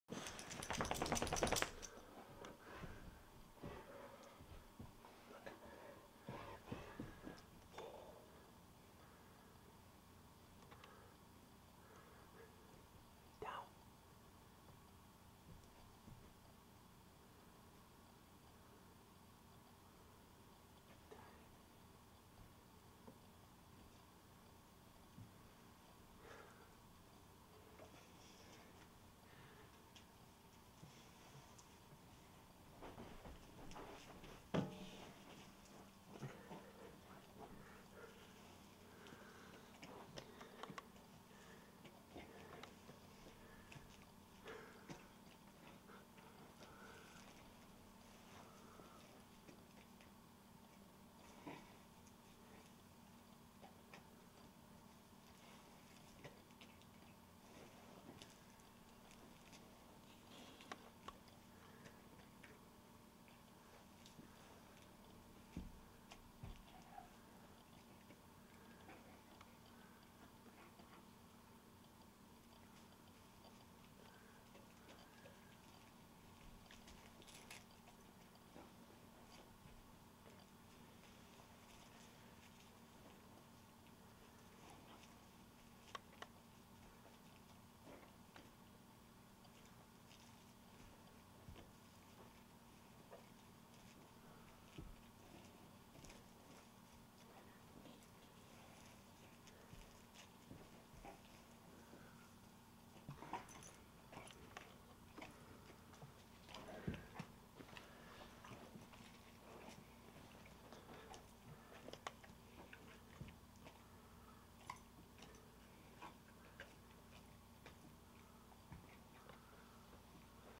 Borzoi Opens His Own Christmas Gift
Korotai The Borzoi finds his christmas gift, and unwraps it and begins eating it.  The image was taken with a panasonic HVX-200 camera, with a redrock micro M2 Adapter using a Cooke Zoom.  Notice the graininess from light loss through the M2 Adapter.